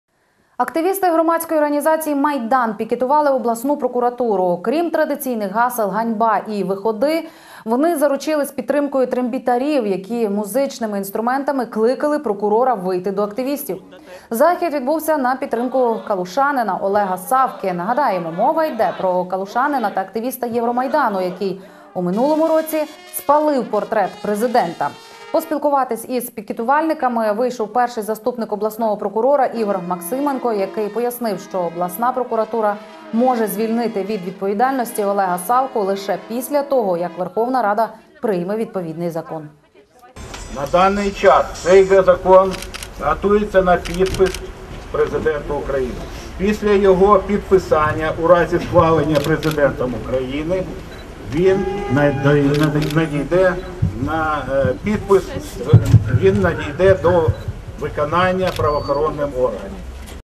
Активісти громадської організації «Майдан» пікетували обласну прокуратуру
Окрім традиційних гасел «Ганьба» і «Виходи!» вони заручились підтримкою трембітарів, які музичними інструментами кликали прокурора вийти до активістів. Захід відбувся на підтримку калушанина Олега Савки. Нагадаємо, мова йде про калушанина та активіста Євромайдану, який у минулому році спалив портрет Президента. Поспілкуватись із пікетувальниками вийшов перший заступник обласного прокурора Ігор Максименко, який пояснив, що обласна прокуратура може звільнити від відповідальності Олега Савку лише після того, як Верховна рада прийме відповідний закон.